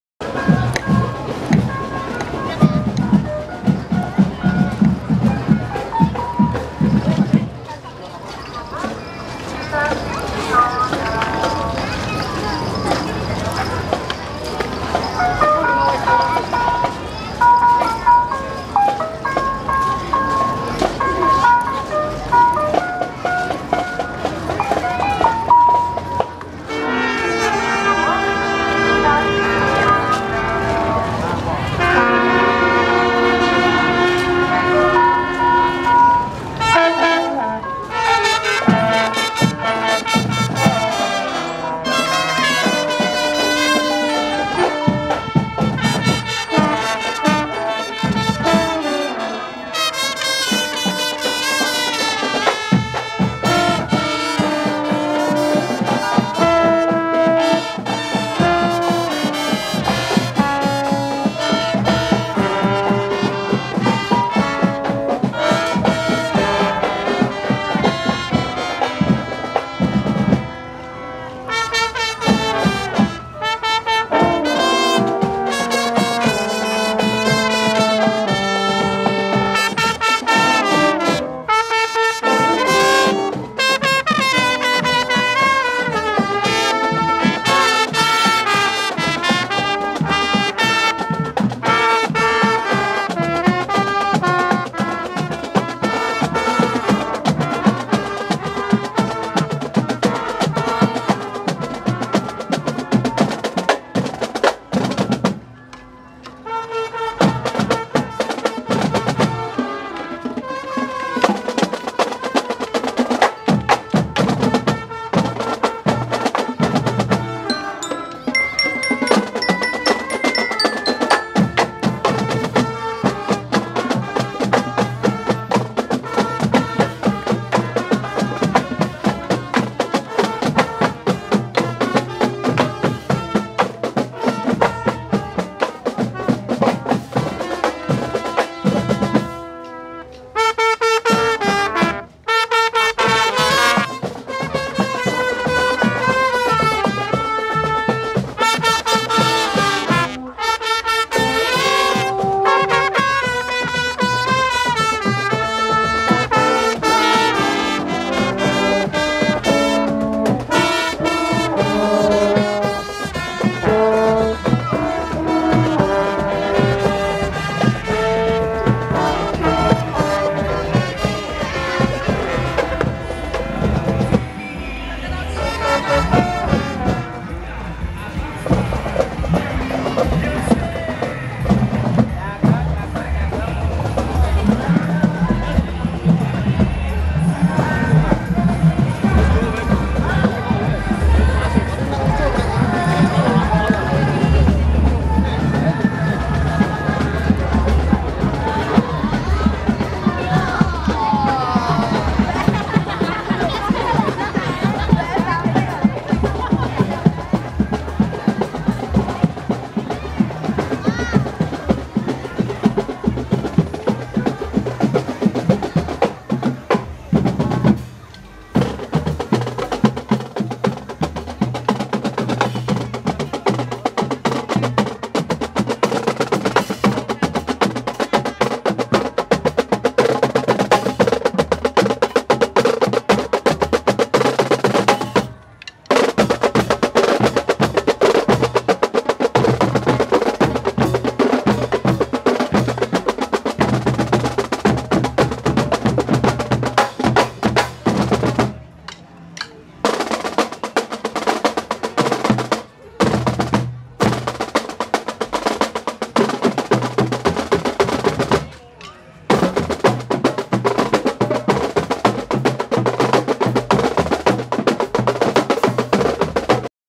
Penonton KAGUM Cewek Cantik Marcing Band MA Walisongo Kayen
ini adalah video karnaval ngemplak sampai Penonton KAGUM Marcing Band MA Walisongo Kayen